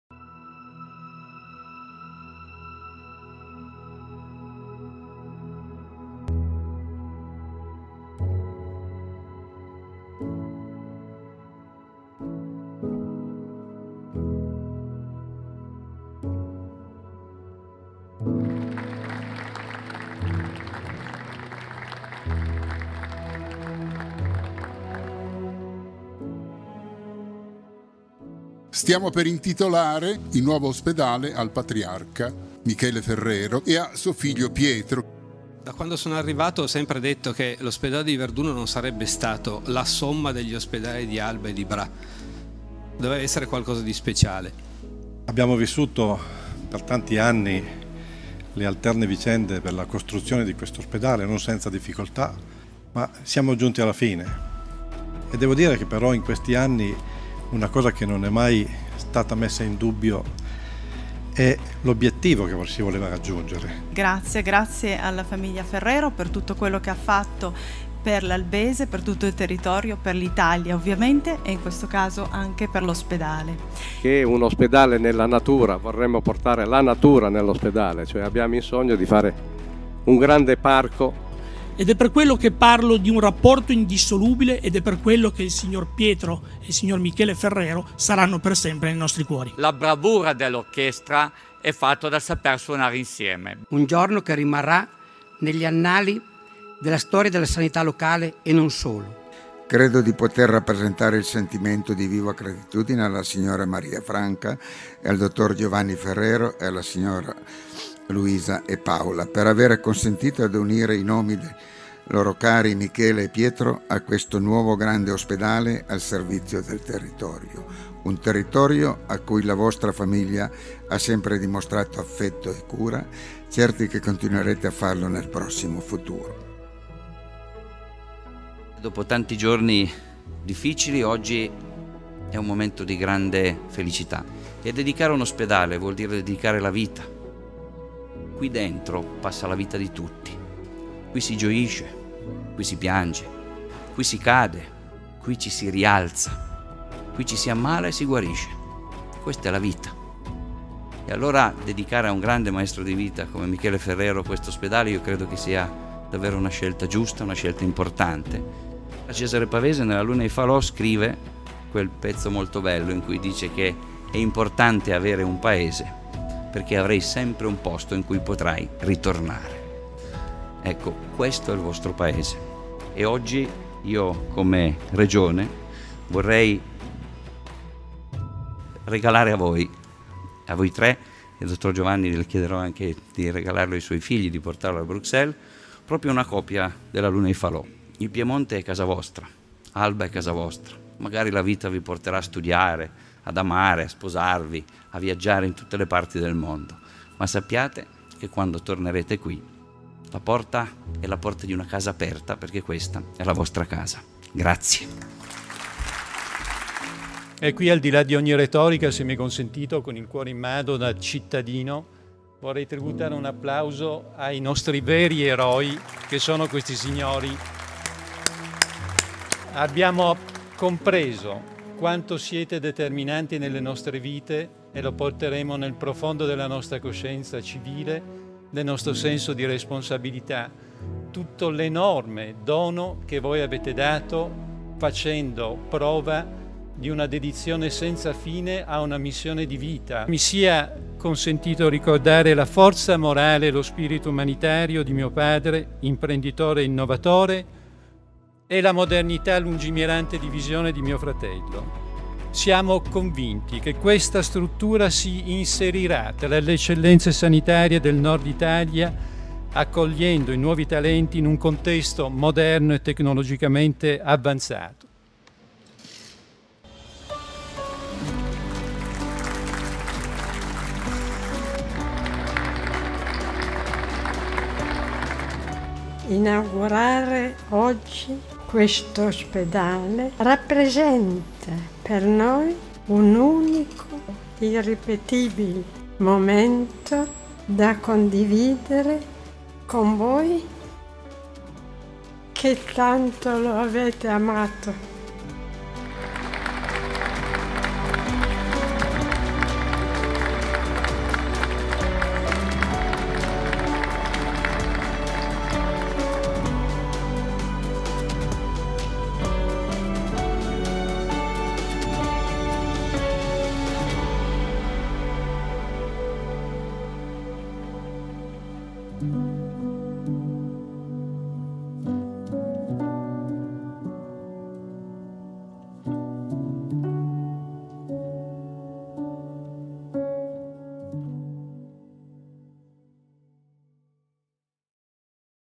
Intitolazione Ospedale Michele e Pietro Ferrero
29 settembre 2020: l’ospedale di Verduno viene intitolato a Michele e Pietro Ferrero. Un segno di sincera riconoscenza da parte di un intero territorio.
Nell'occasione, è stato annunciato un  nuovo progetto: la creazione di un parco intorno all'ospedale che partendo dagli spazi interni, dai cortili, dai terrazzi, dalle aiuole si collegherà con l’esterno, dando vita a un vero e proprio healing garden, un giardino curativo, a disposizione di pazienti, personale sanitario, famiglie in visita. In particolare, i bambini ricoverati potranno uscire, muoversi e godere di spazi all'aperto, fondamentali per ritrovare il benessere e l’equilibrio psicofisico.
La realizzazione di questo straordinario e composito parco sarà ancora possibile  grazie alla Famiglia Ferrero, che ha annunciato di voler donare e dedicare l’intera opera al ricordo di Michele e Pietro Ferrero.